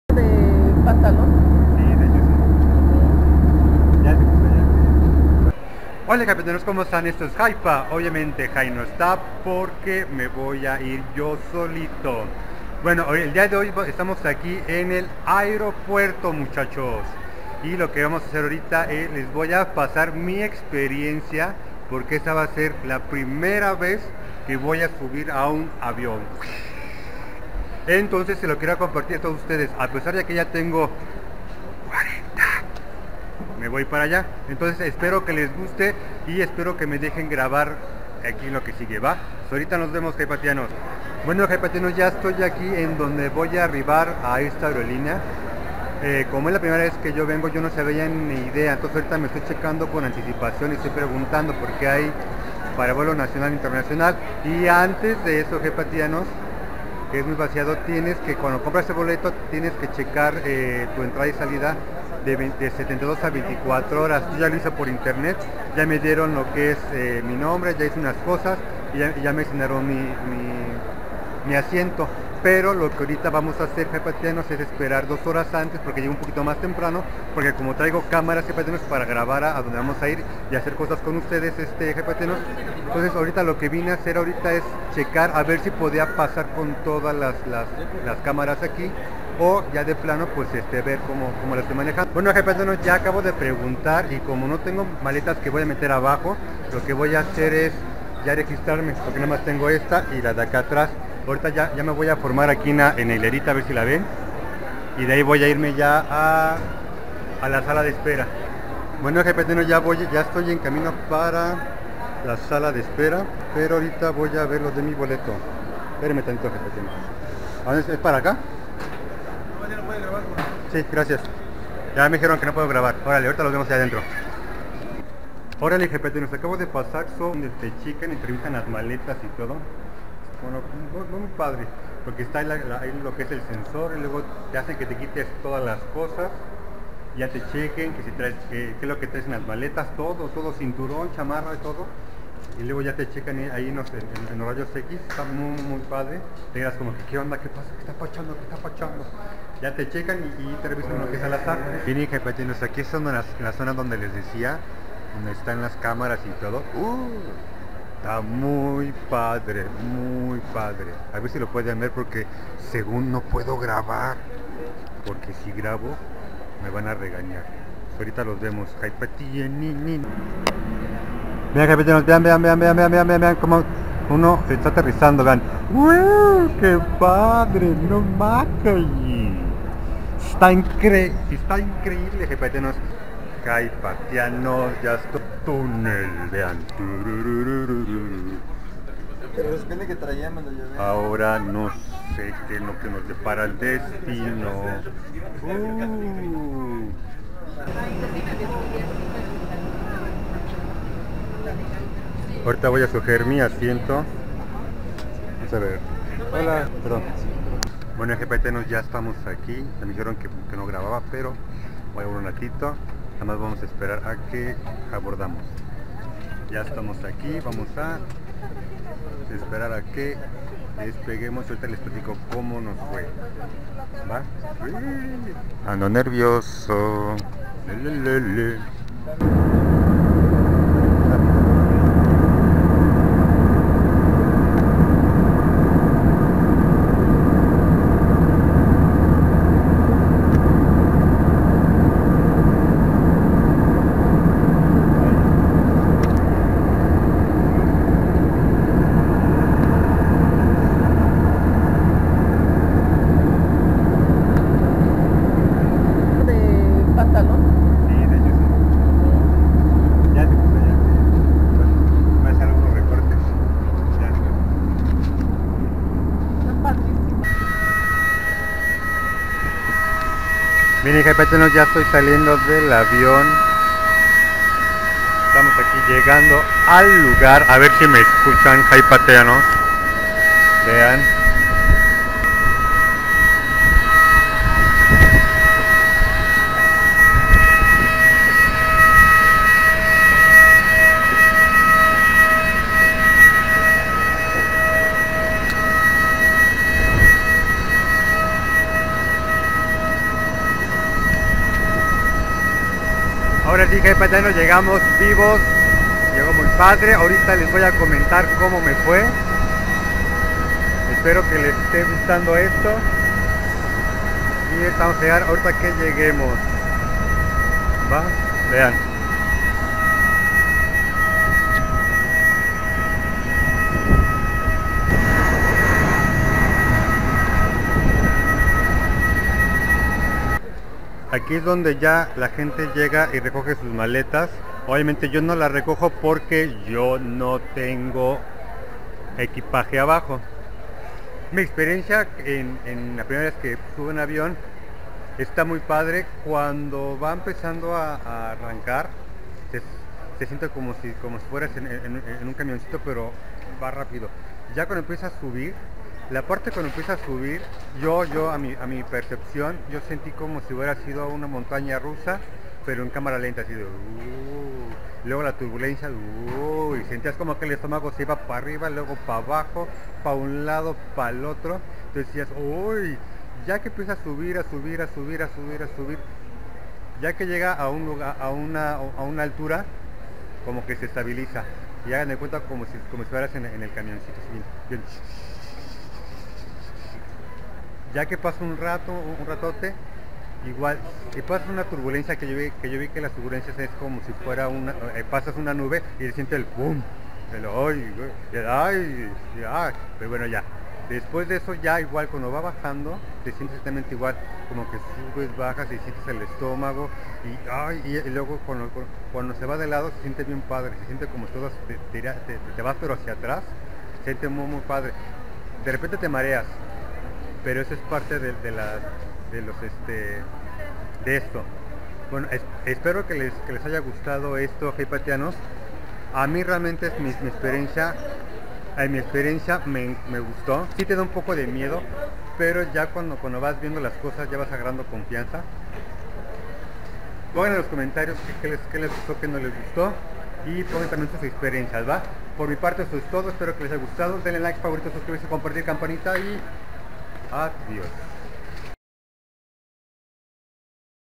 PRIMERA VEZ EN AVIÓN A LOS 40 AÑOS I HIPA MEXICO
Queria compartir mi PrimeraExperiencia o PrimeraVez en SubirmeAUnAvion, hipatianos, lo único que te da miedo es cuando despegas y cuando a terrizas, pero lo demás esta increíble.